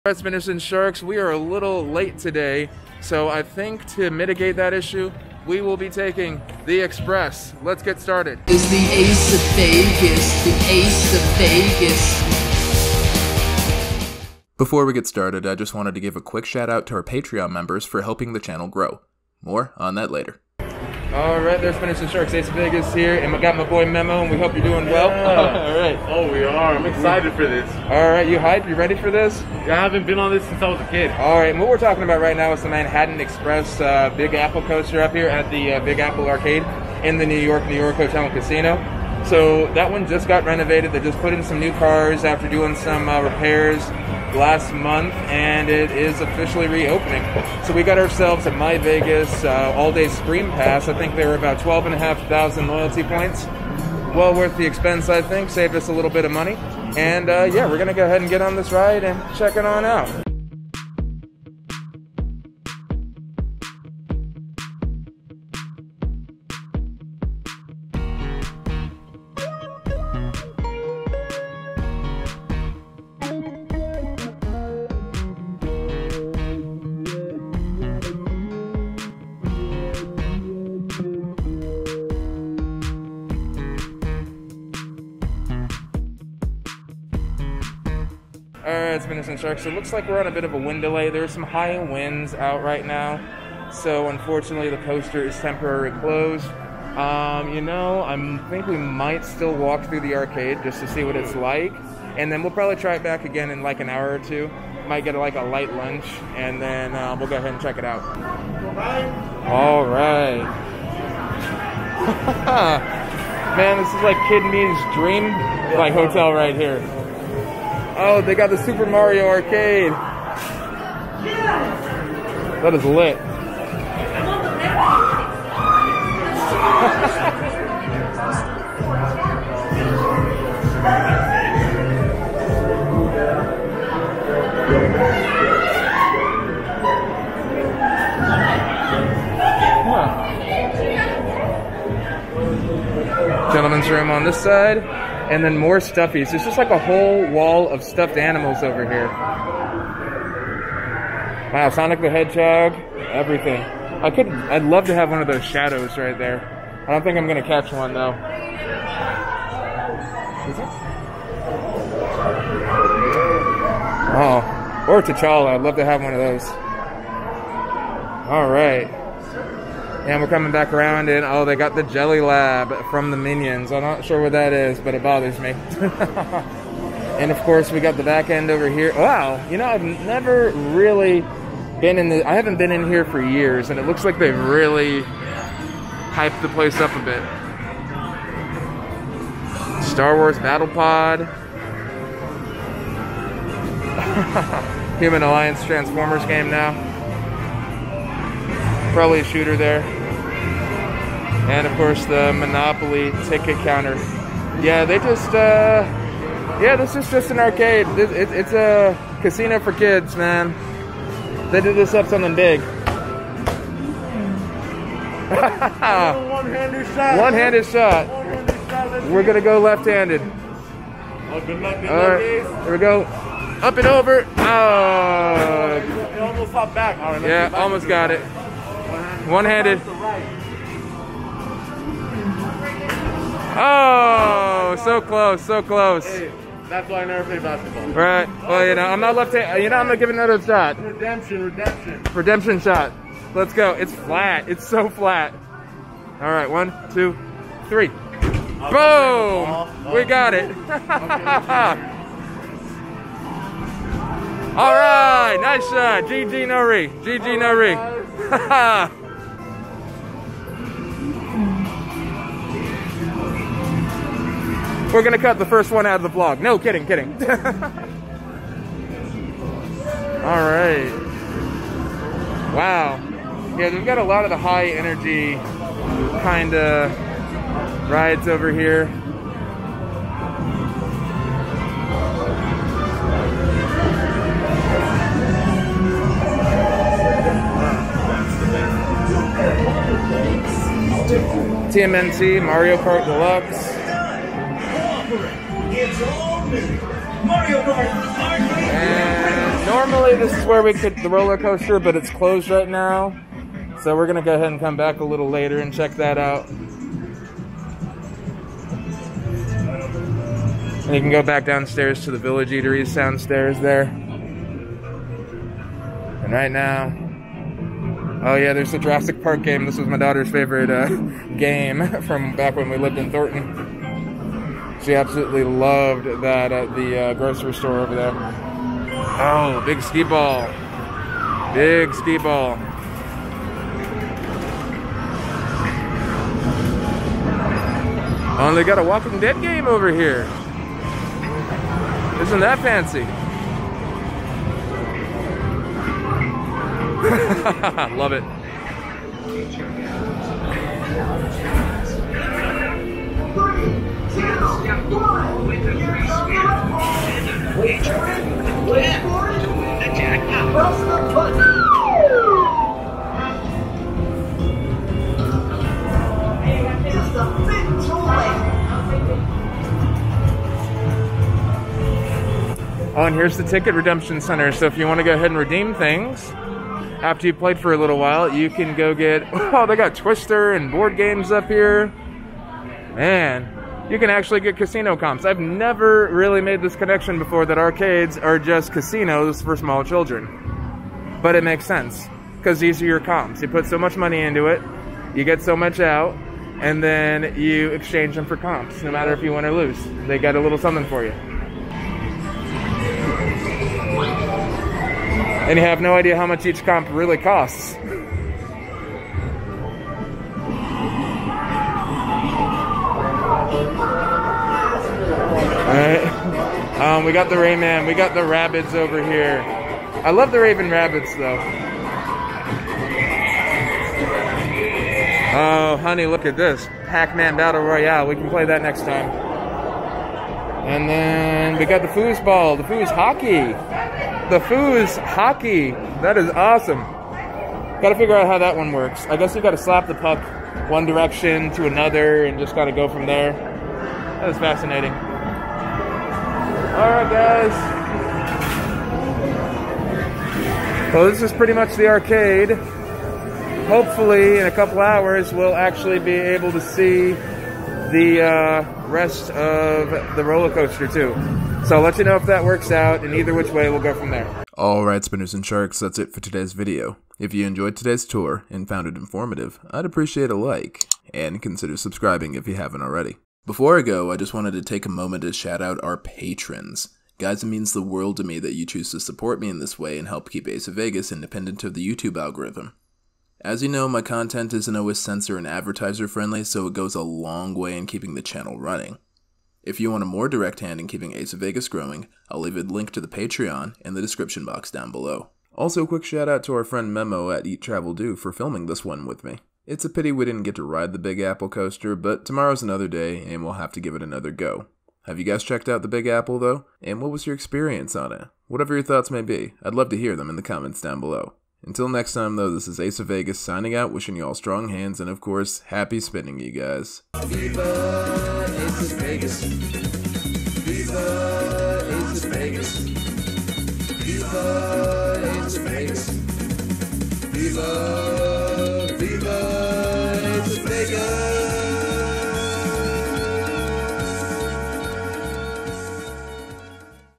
NYNY Big Apple Arcade Tour 4K Las Vegas
shout out to Eat Travel Doo for filming this with me. We're taking a 4K look at the NYNY Arcade in Las Vegas. We didn't get to do the coaster though, dang weather delays :(

And if you're still here, check out Ace's much more family friendly channel Battle Master Ace for Pokemon Challenges